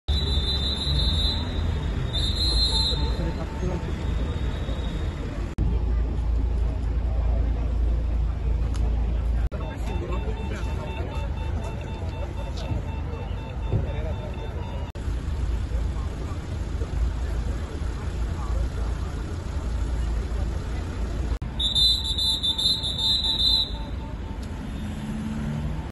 Accident in Cluj cu Ambulanta, un autobuz si un autoturism 6 11 2024
Un accident rutier în care au fost implicate trei autovehicule s-a produs miercuri seara, la Cluj-Napoca. vezi mai mult aici